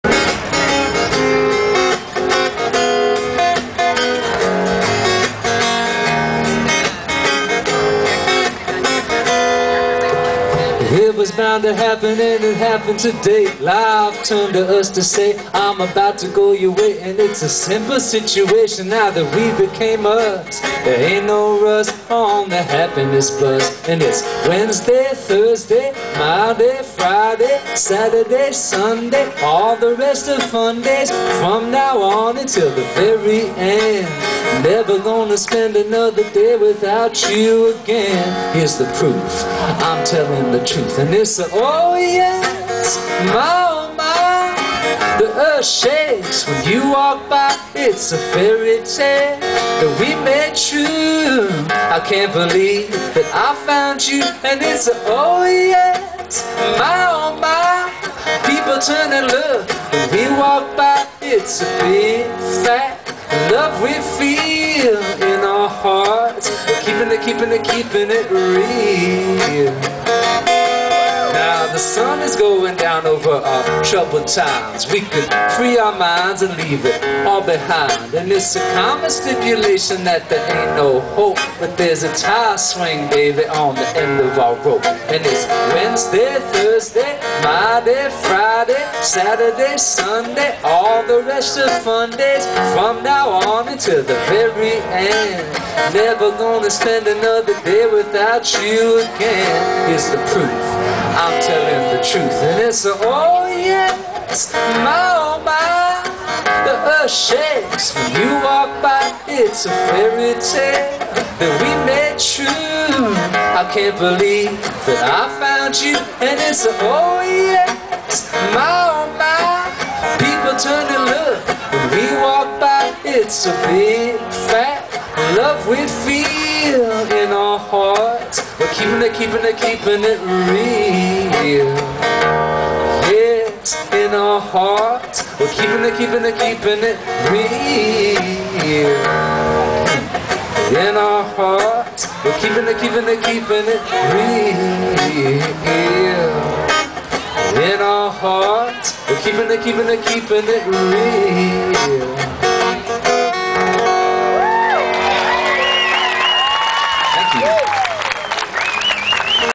Mason Jennings August 2, 2008 Kidzapalooza
Mason Jennings live at the kids stage, day 2 of lollapalooza 2008.